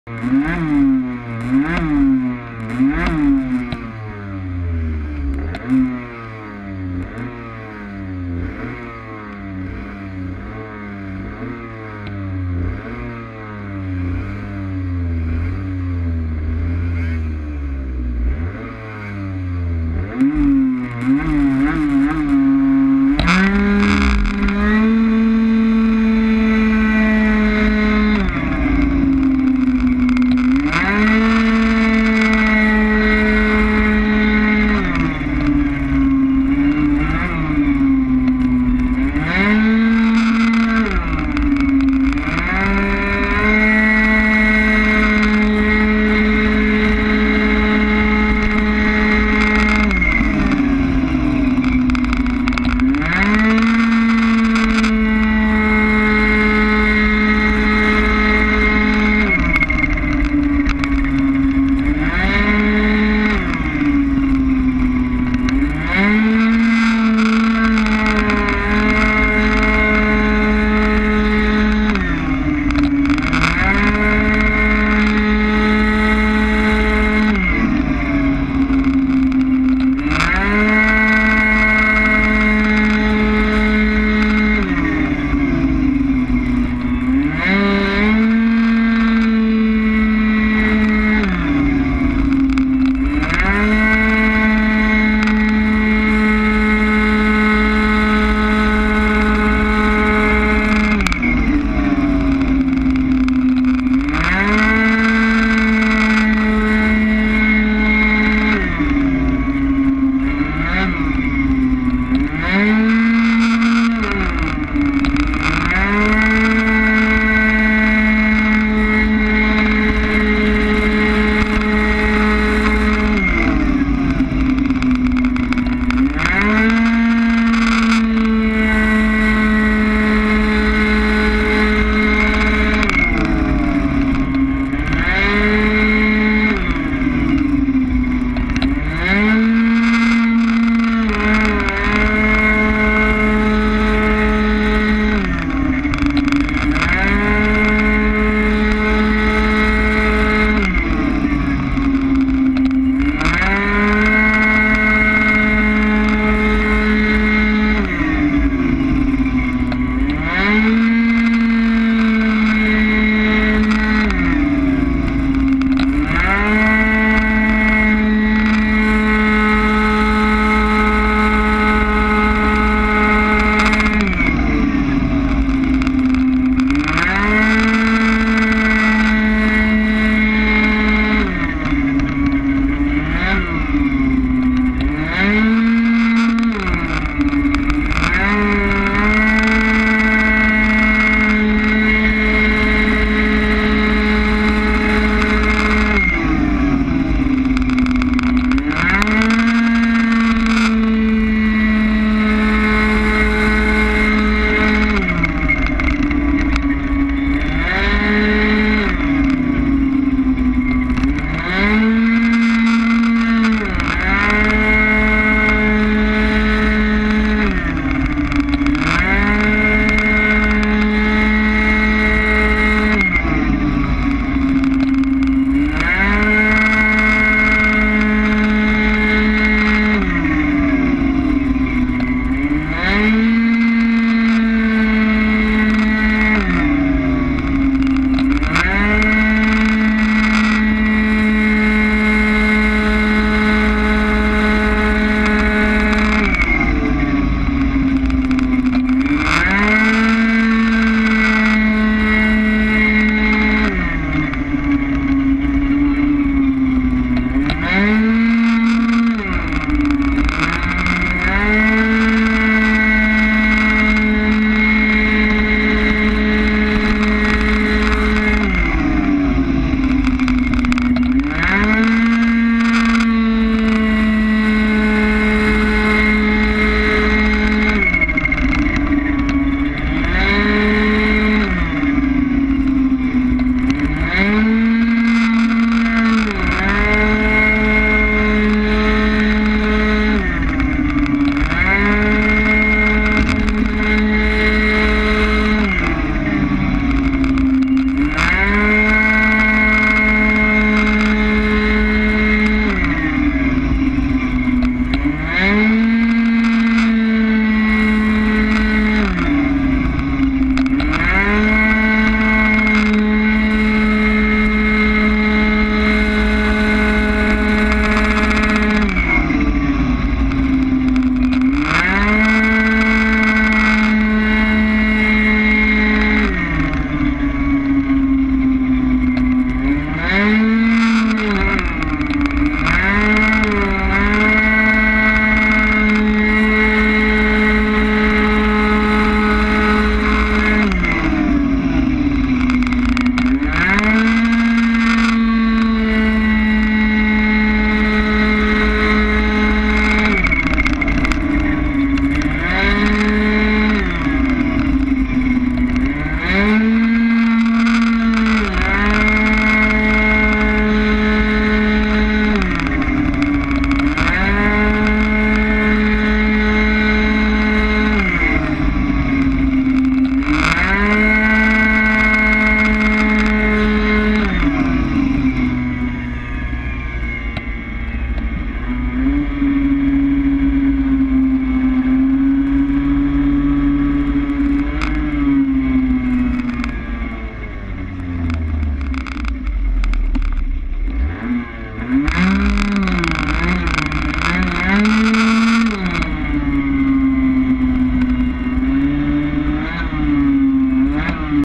Manche 2/1 TGO GoPro Avant#72 Piste de Ancenis Dimanche 21 Septembre 2014
Grosse Chute #3 Promo Vario Arrêt de la Course
Pilote Julien Malaguti F12 #72  Promo Scoot 50cc Metrakit